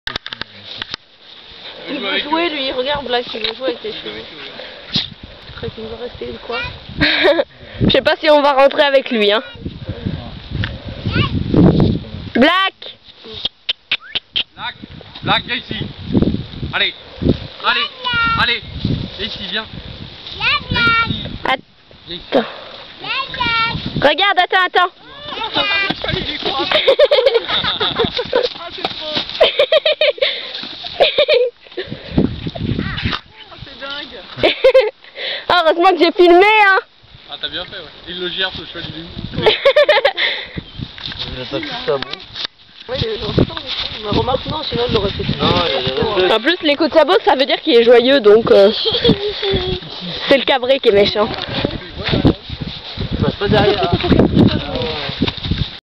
Black qui joue avec des chevaux .
Black qui joue avec deux chevaux sauvé de la boucherie ...